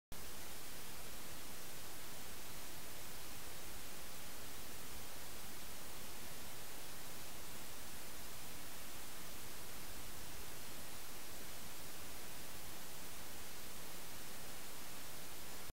Snowy Owl Focus
The movement is ms snowy spotting something